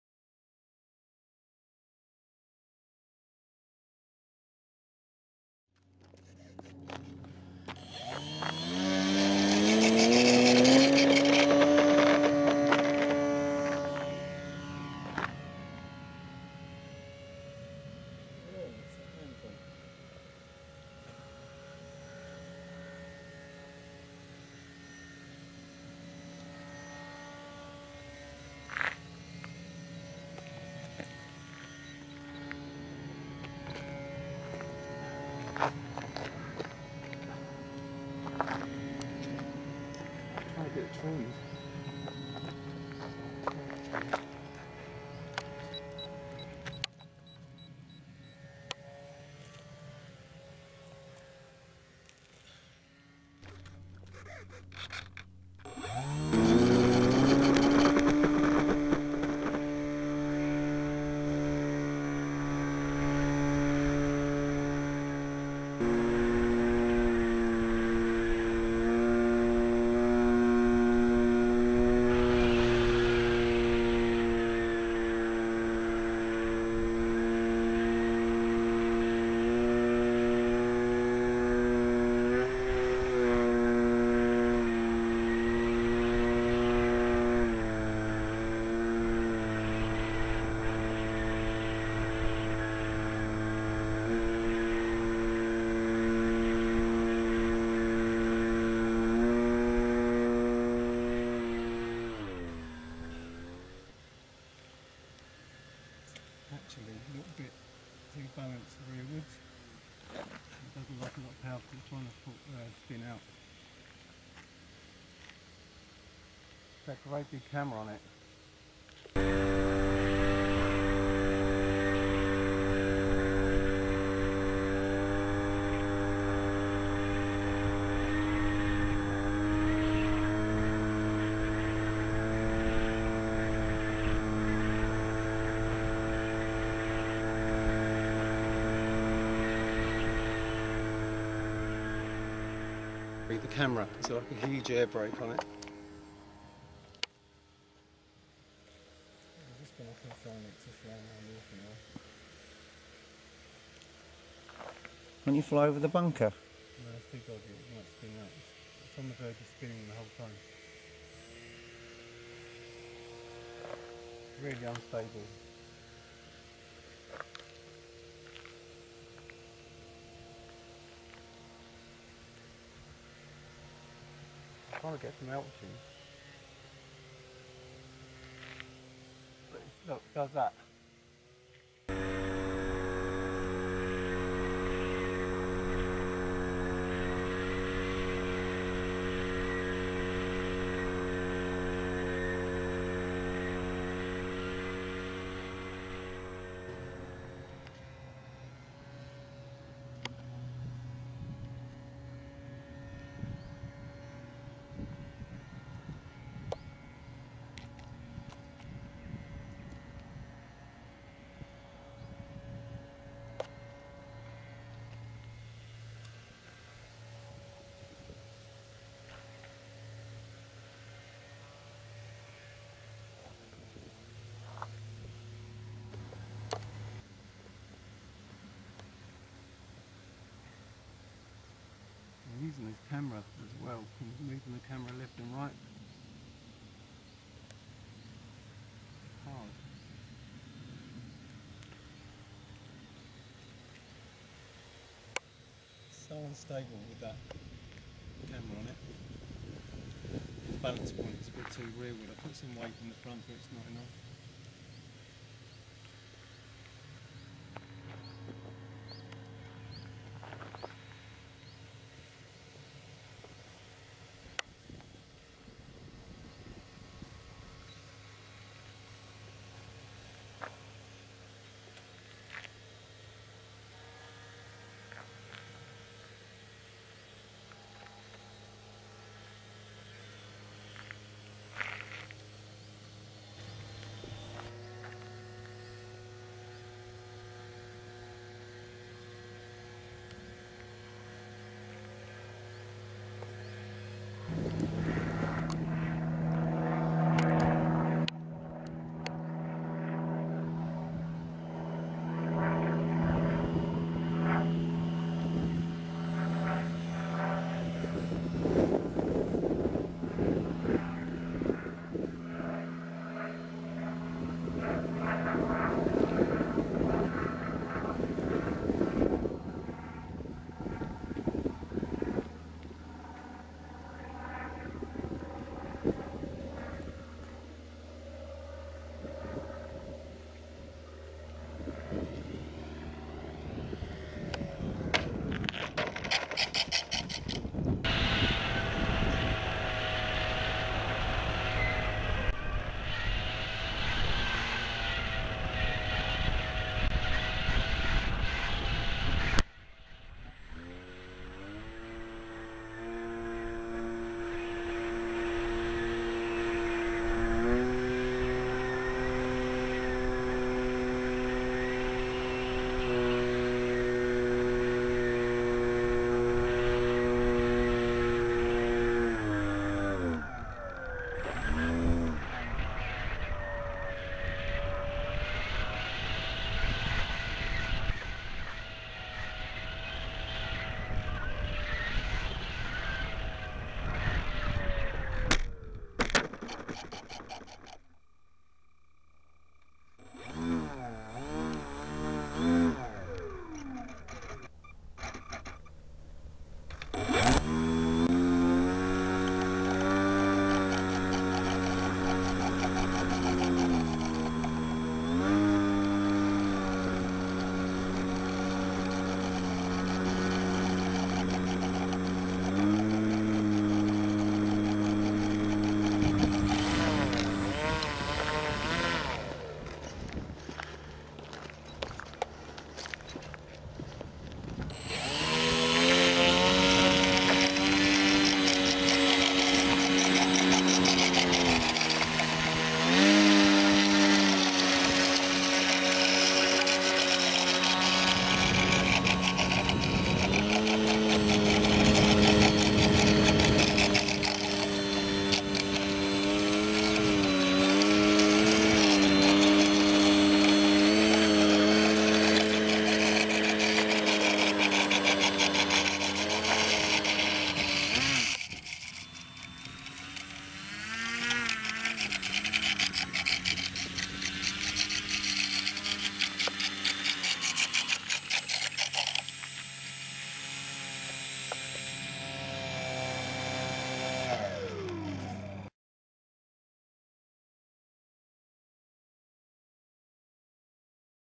Green models Tiger Moth on first flight after rebuild, in HD
Hre's my Moth with a Hero GO PRO HD cam on board. The box between the cabanes houses the camera, it was a fairly dicey flight due to the extra weight and some trimming issues.